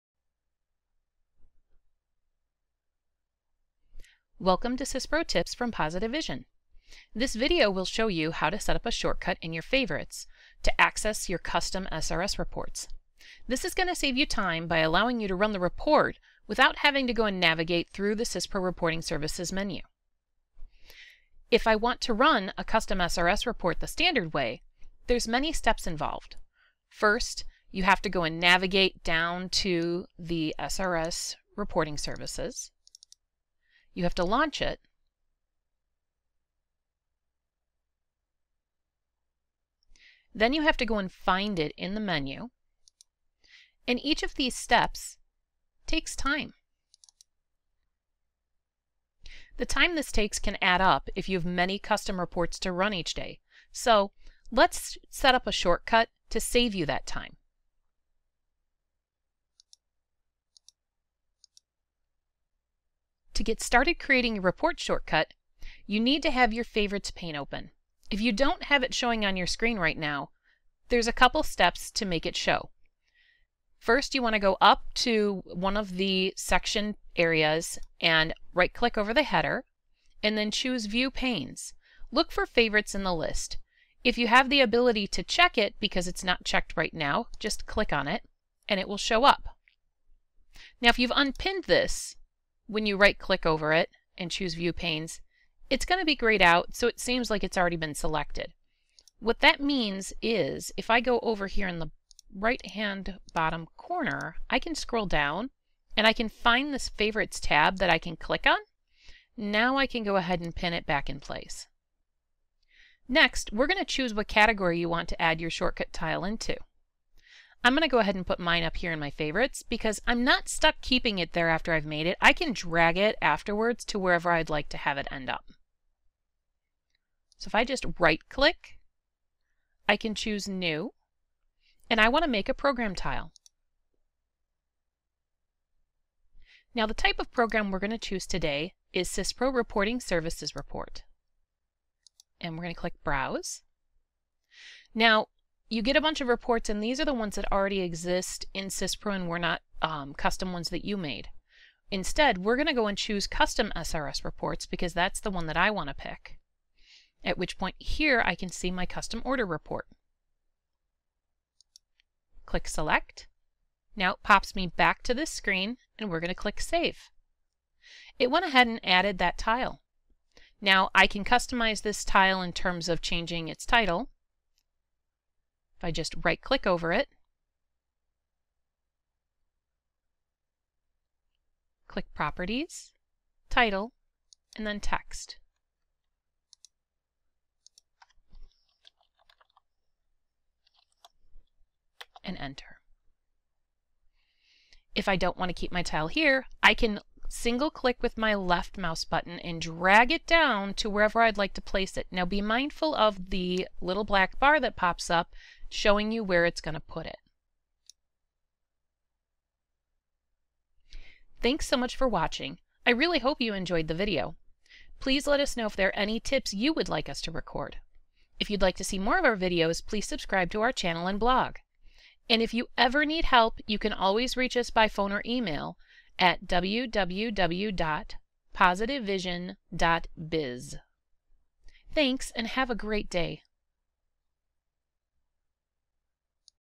SYSPRO Tips - How to Set Up a Shortcut to Your Custom SRS Reports in SYSPRO
This video will teach you how to set up shortcuts to quickly access your custom SRS reports in SYSPRO.